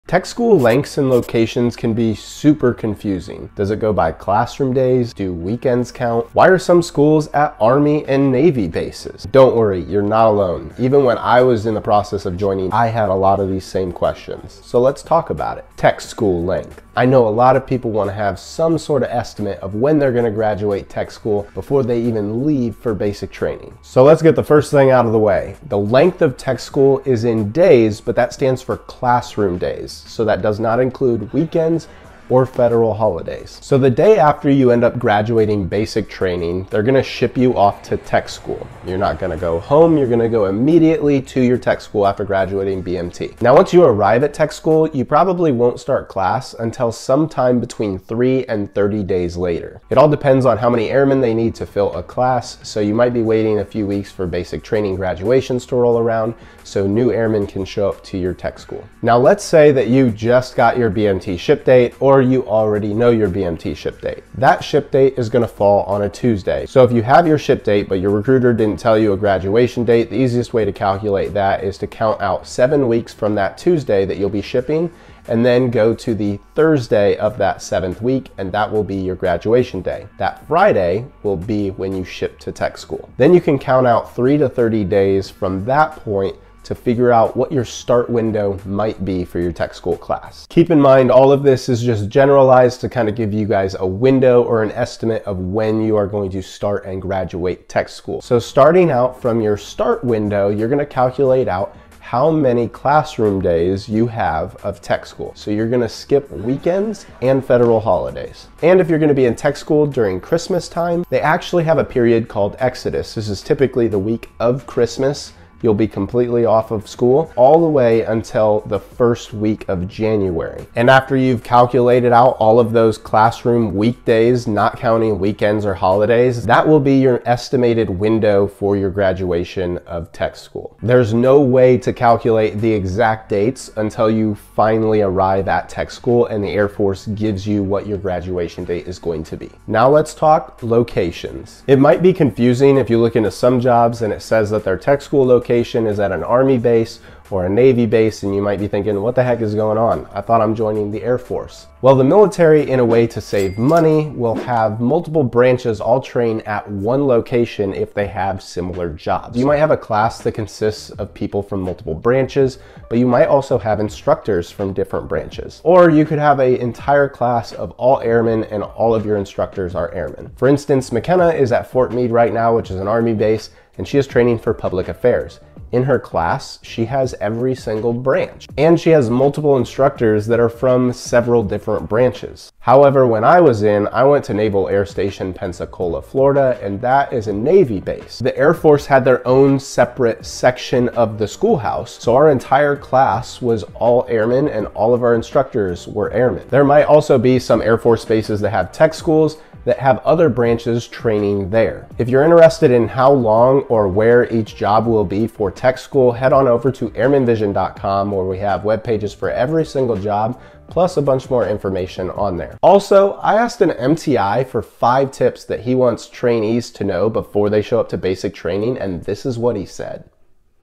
Air Force Tech School - Length and Location explained
Where is my Tech School and how long will I be at Tech School are 2 of the most asked questions we get about Tech School here on Airman Vision. We not only answer those two questions but we also share with you how to estimate your Tech School graduation date. So if you've ever wondered, when will you graduate Tech School? This video will help answer that as well!